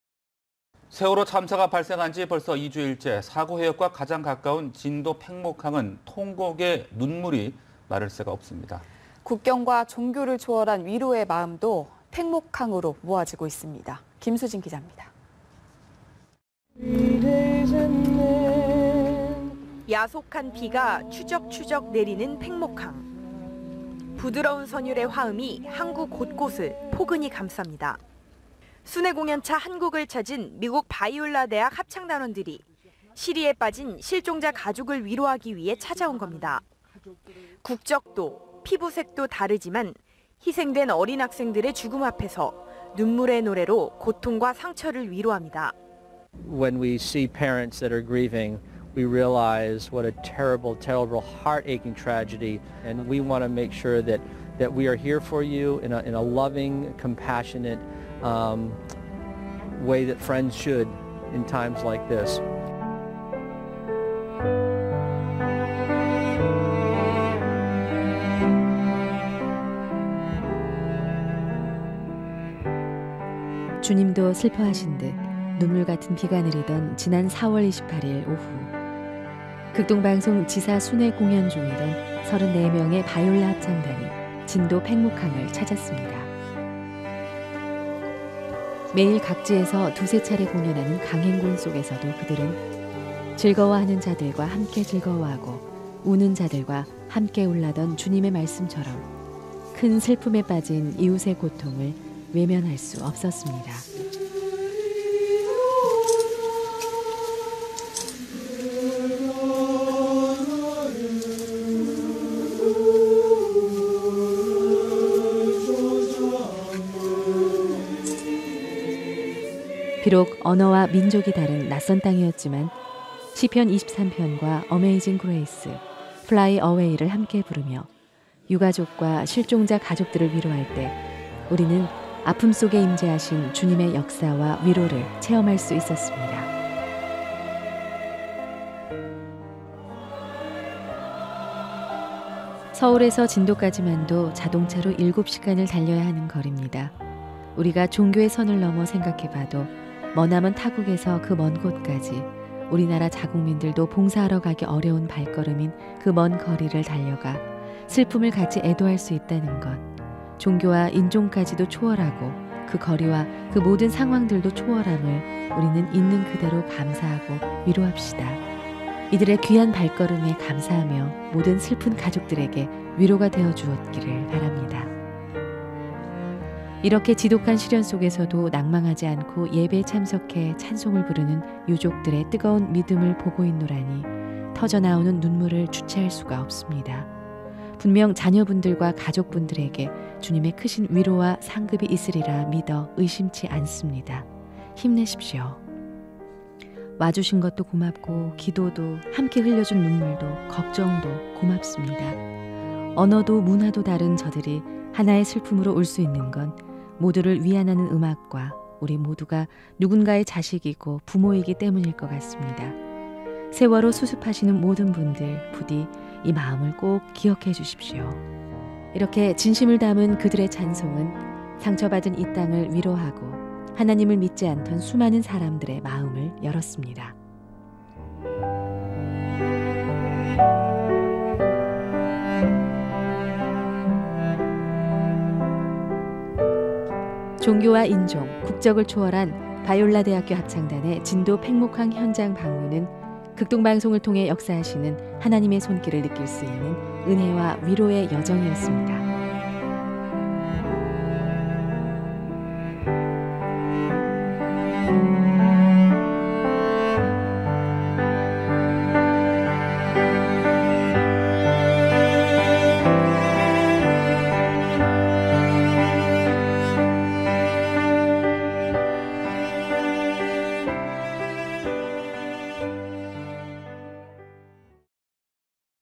Biola University Chorale Visits South Korea
Biola University visits Jindo, South Korea to mourn with families and victims of the ferry accident.